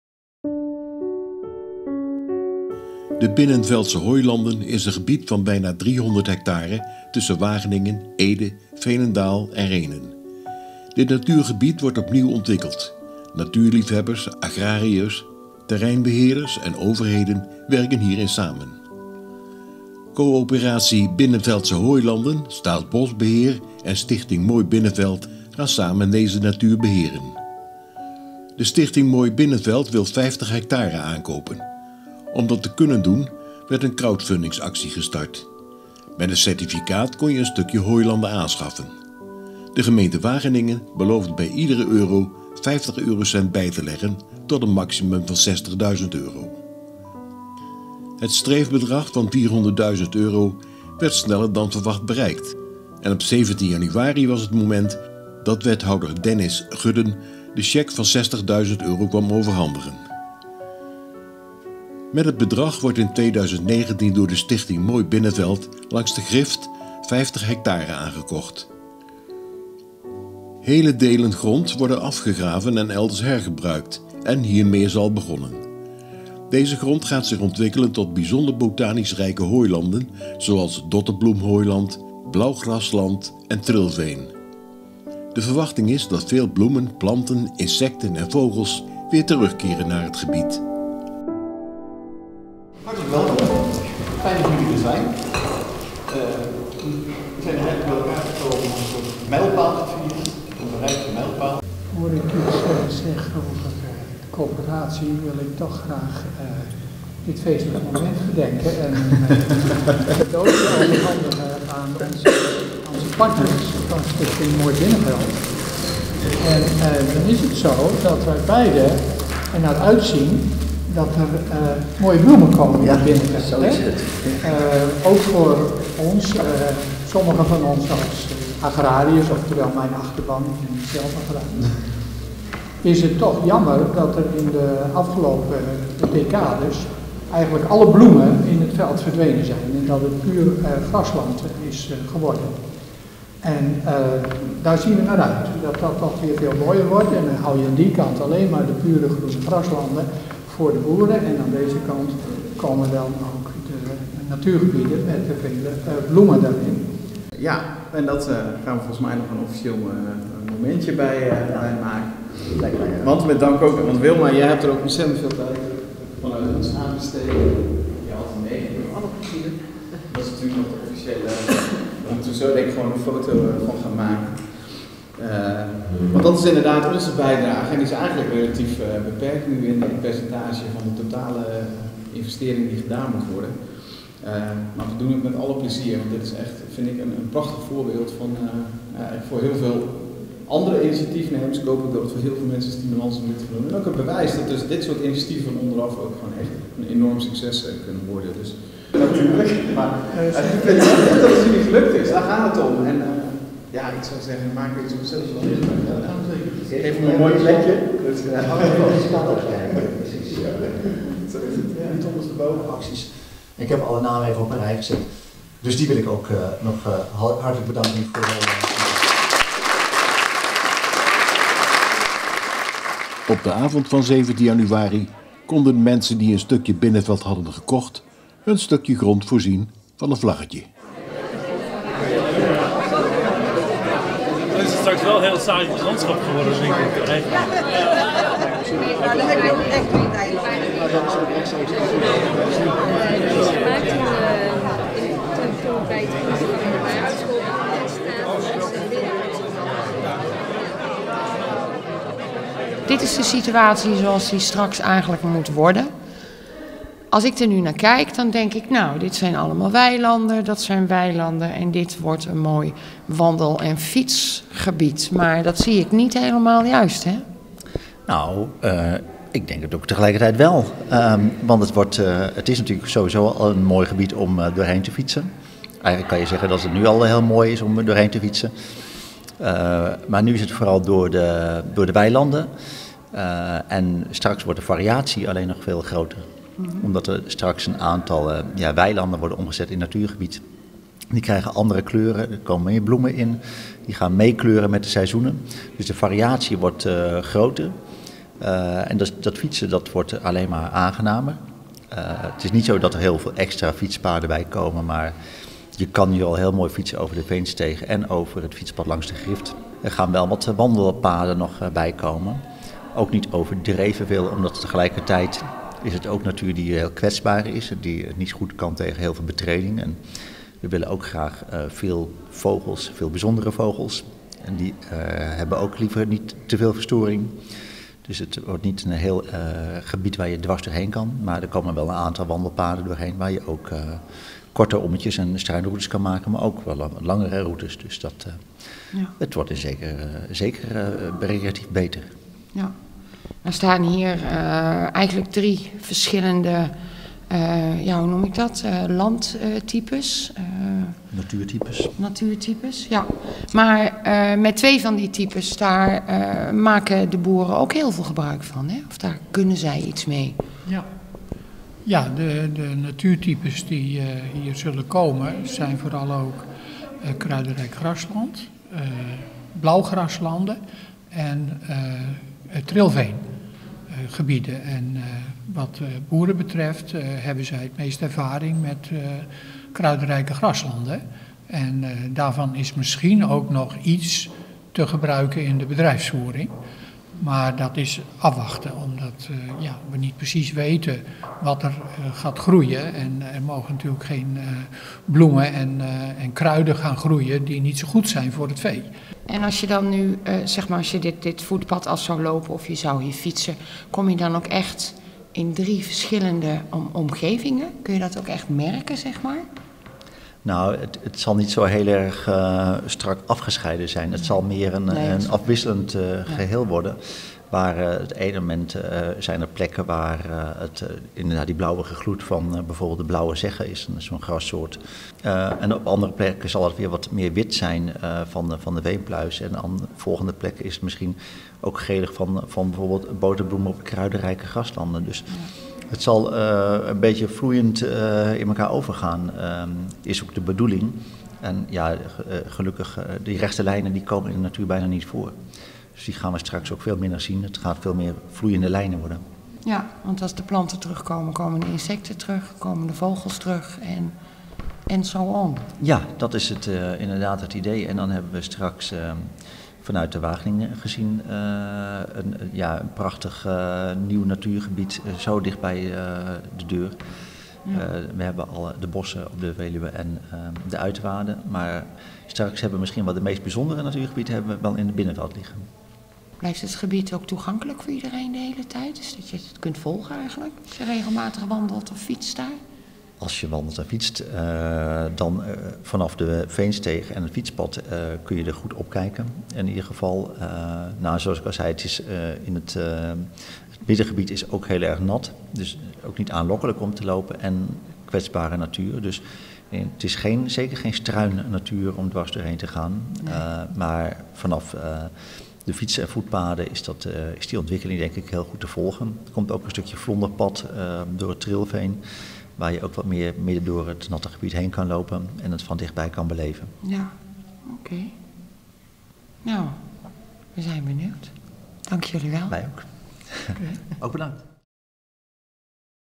Mooi Binnenveld
De Binnenveldse Hooilanden is een gebied van bijna 300 hectare tussen Wageningen, Ede, Veenendaal en Rhenen. Dit natuurgebied wordt opnieuw ontwikkeld, natuurliefhebbers, agrariërs, terreinbeheerders en overheden werken hier samen aan.